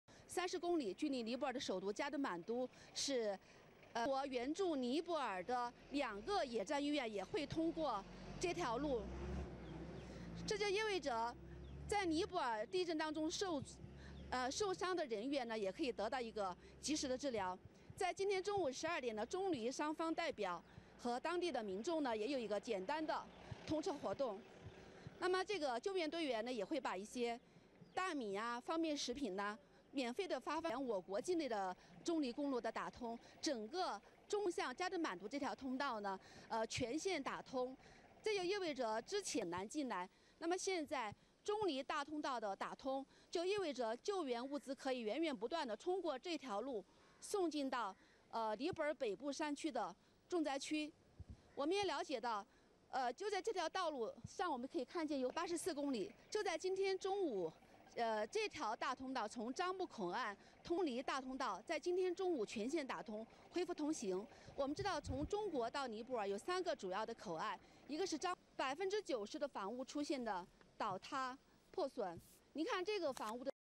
China-Nepal Highway Blocked in Quake Reopens
China's armed police force restored a cross-border highway linking earthquake-hit Nepal with China at noon on Sunday.

The pivotal China-Nepal highway, which was built in 1965, is a scenic route connecting Lhasa City in southwest China's Tibet with the Nepalese capital of Kathmandu.

Along with the reopening of the highway, the delivery of relief supplies from China to Nepal's worst-hit areas will soon be resumed.

Two field hospitals from China will be moved to the Himalayan country on Monday.